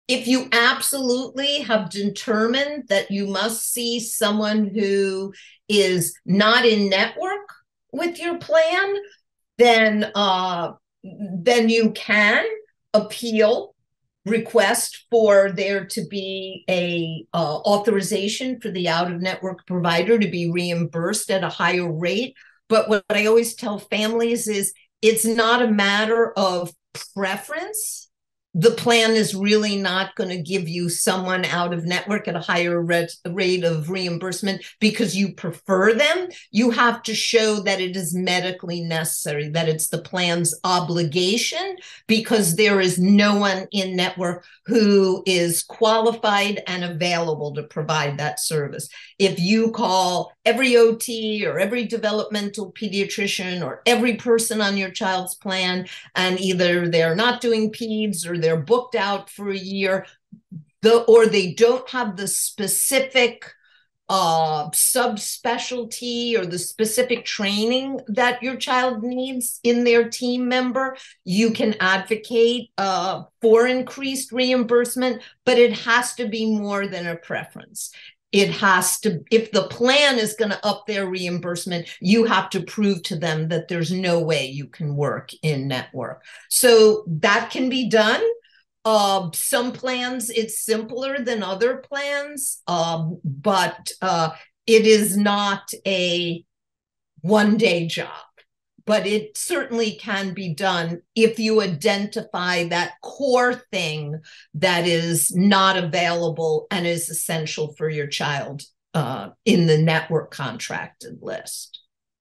What If a Provider You Want Is Out-of-Network?
Is a provider you want out-of-network? You may be able to get an out-of-network provider if you prove that it's medically necessary. Undivided's Director of Health Plan Advocacy, Leslie Lobel, explains how the process works.

For more information about wanting a provider out-of-network, check out our article